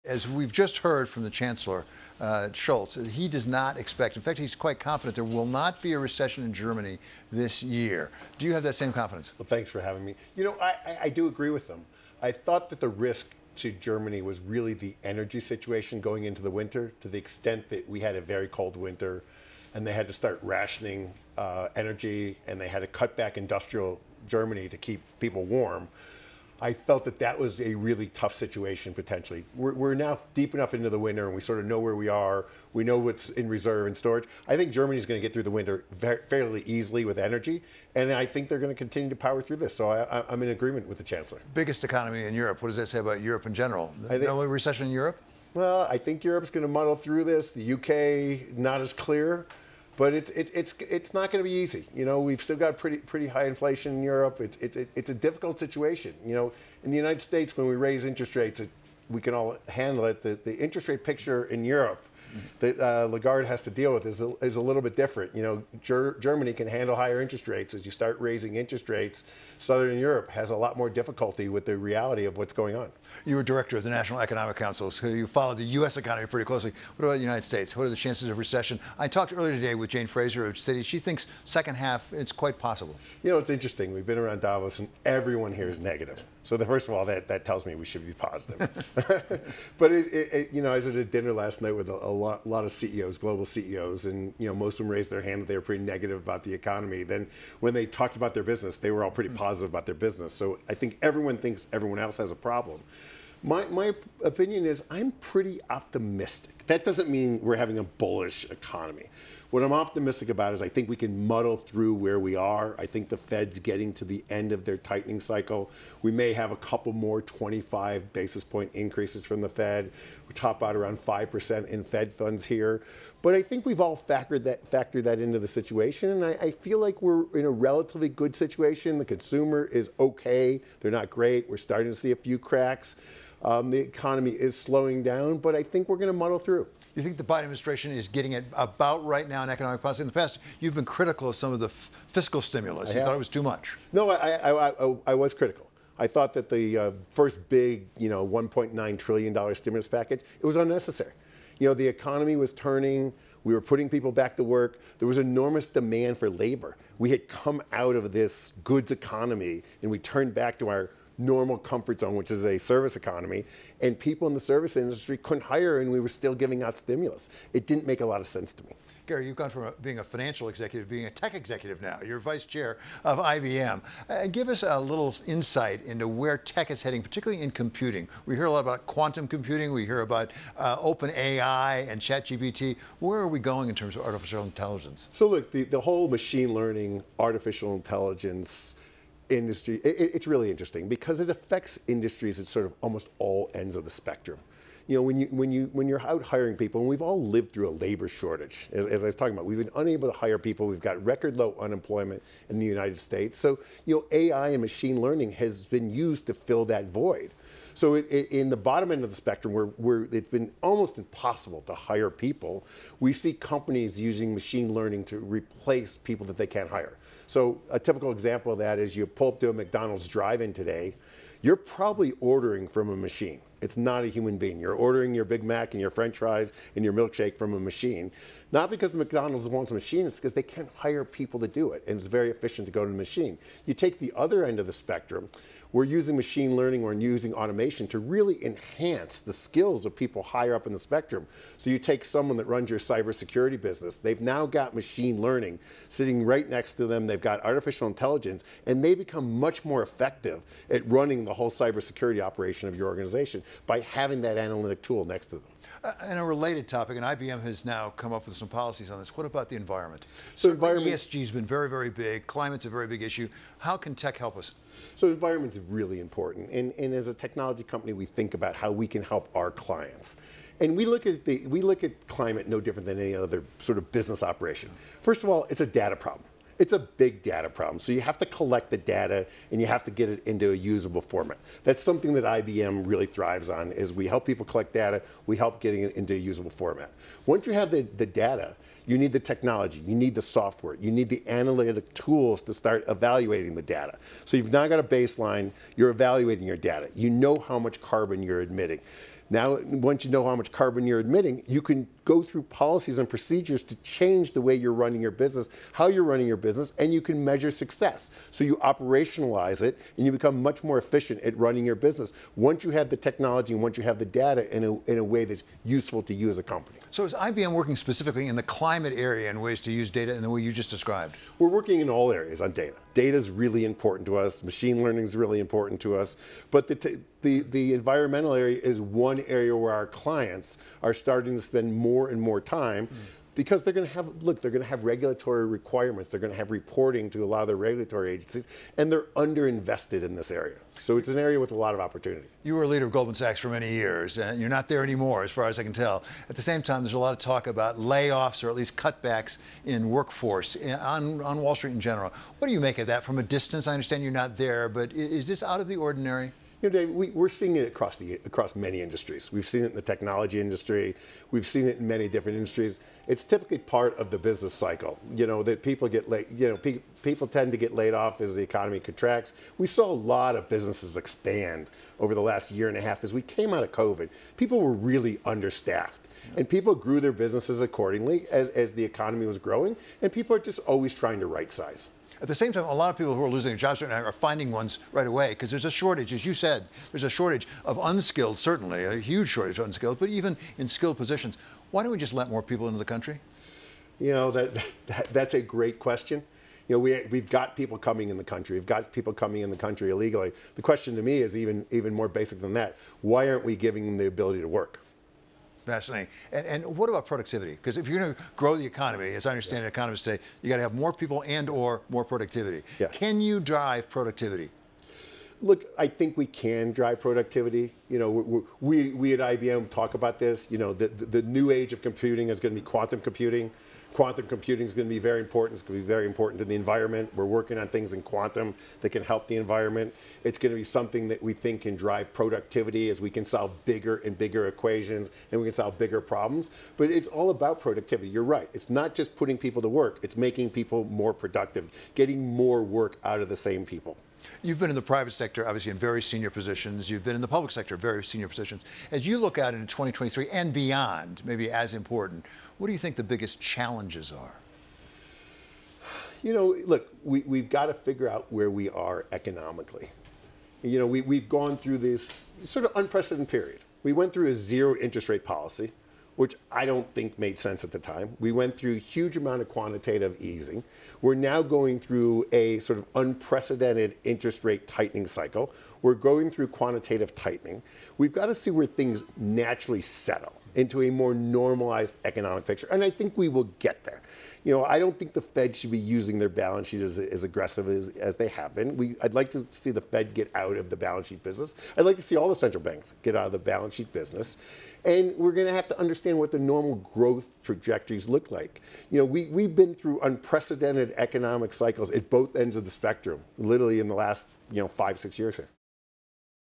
Gary Cohn Is Still Optimistic About US Economy
Gary Cohn, a former economic adviser to President Donald Trump and the current IBM vice chairman, talks about the chances of a recession in the US, changes in technology and layoffs on Wall Street. He speaks to David Westin at the World Economic Forum in Davos, Switzerland.